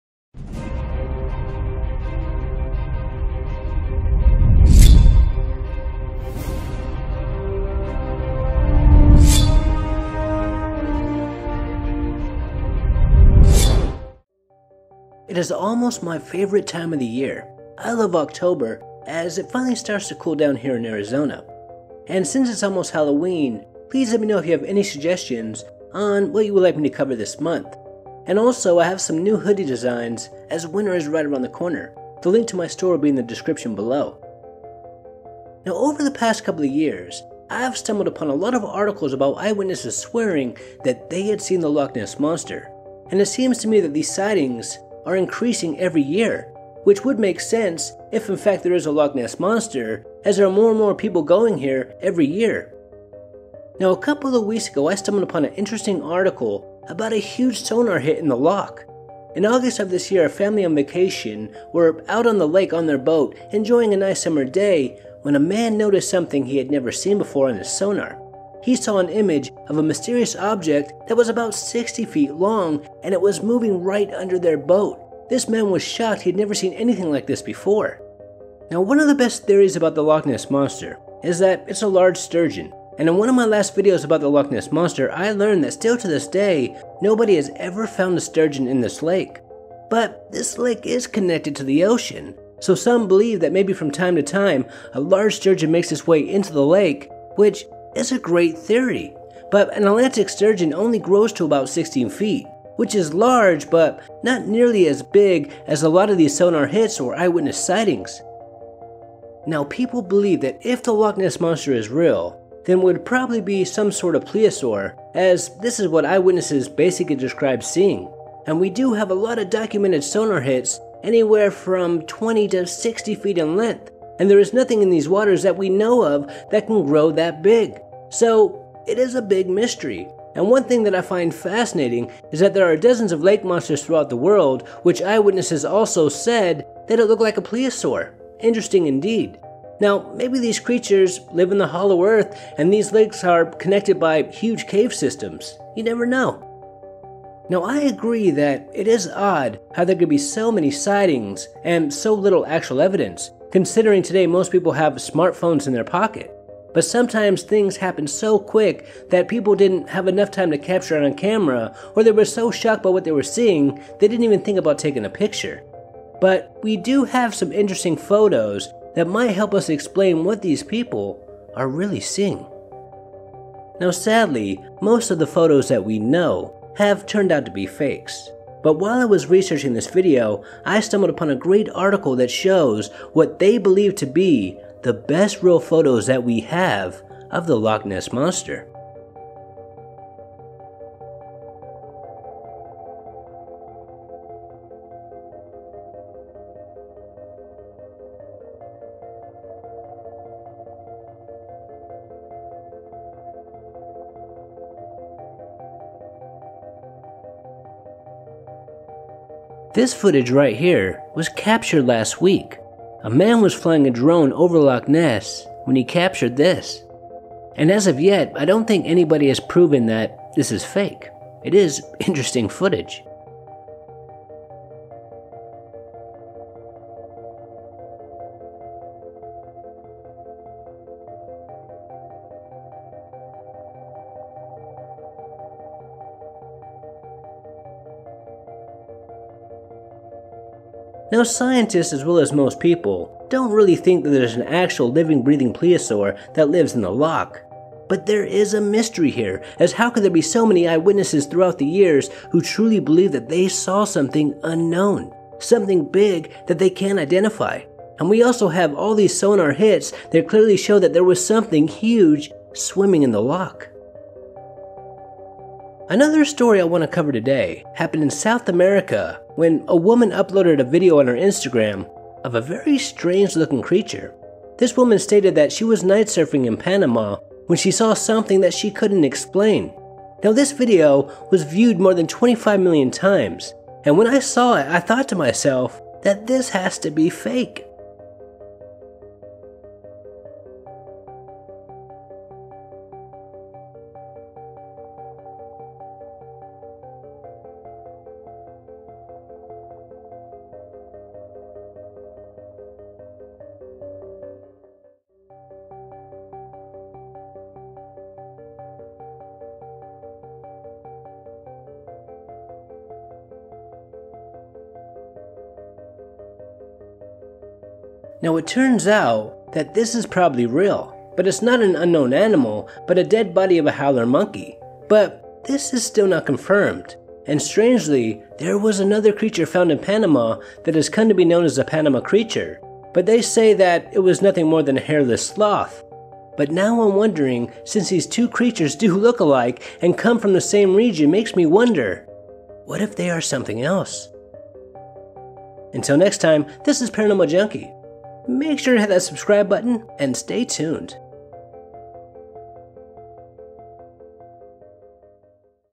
Massive Sonar Hit At Loch Ness | Strange Creature Seen In Panama
Man sees something very large on his sonar at Loch Ness